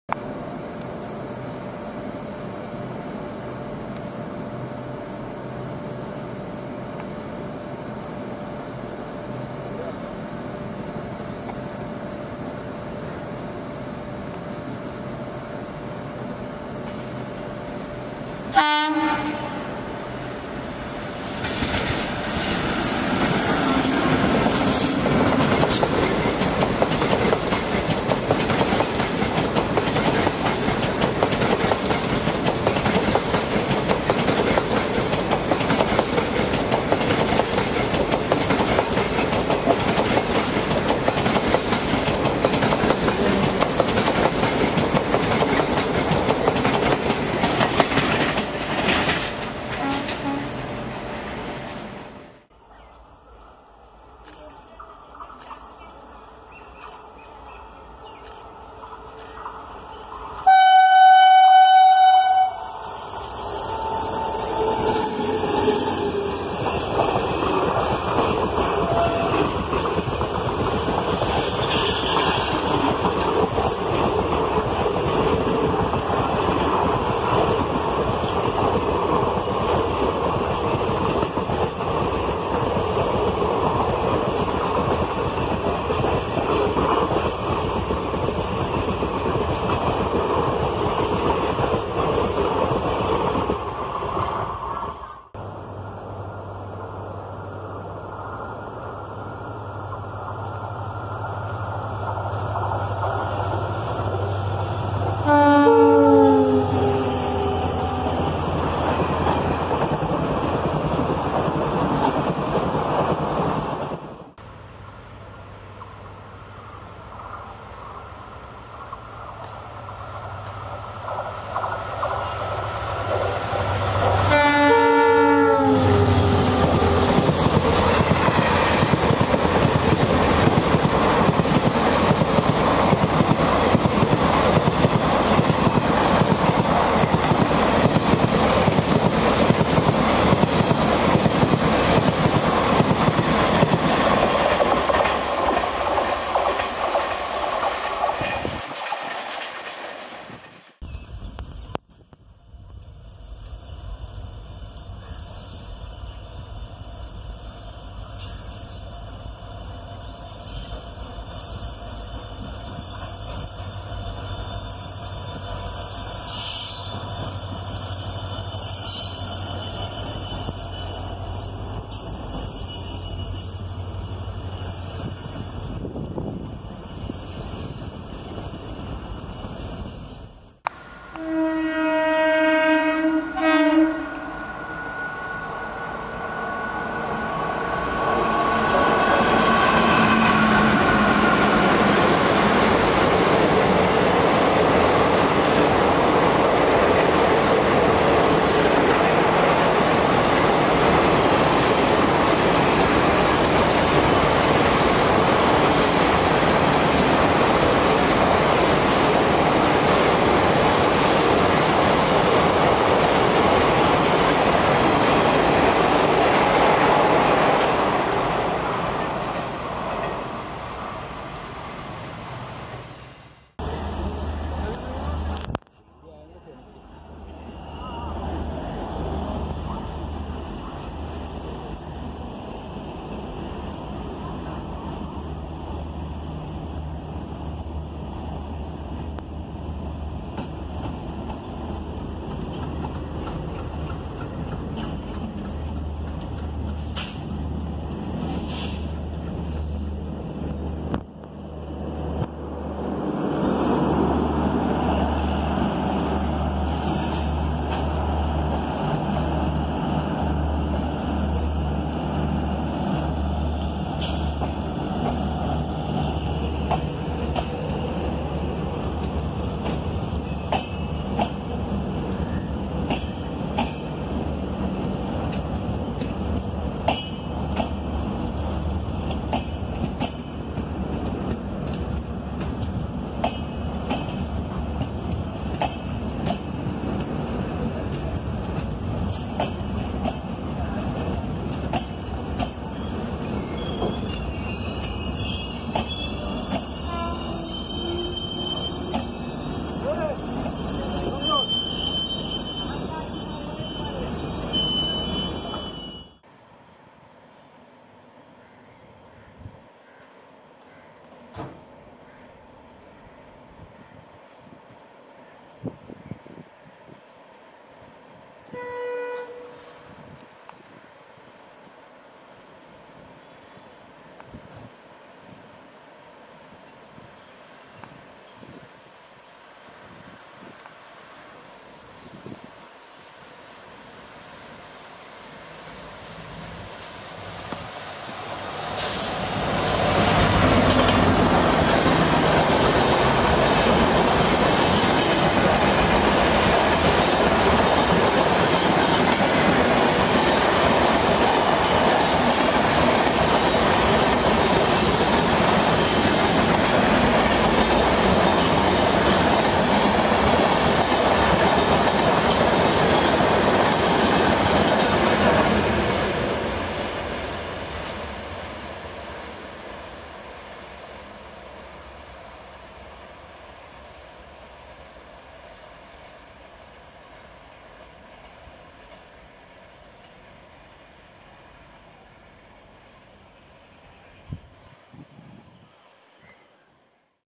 [7 in 1] Kerala Exp, Duronto, Intercity And More
Index:

1. Guruvayur ~ Ernakulam Passenger (ED WAP 4) -- Kalamassery Station

2. New Delhi ~ Thiruvananthapuram Kerala Express (AJNI WAP 7) -- Near Chottanikkara Road Station

3. KSR Bangalore ~ Ernakulam Intercity Express (KJM WDP 4D) MPS Skip (110 KMPH) -- Ottappalam Station In two angles

4. Okha ~ Ernakulam Express (GOC WDP 4D) -- Uduppi Station

5. Alappuzha ~ Chennai Central Superfast Express (ET WAP 4) -- Near Kalamassery Station

6. Mumbai Lokmanya Tilak ~ Ernakulam AC Duronto Express (KYN WDM 3D Twins) -- Ernakulam Junction Station

7. Mumbai Lokmanya Tilak ~ Kochuveli Superfast Express (KYN WDP 4D) -- Thrippunithura Station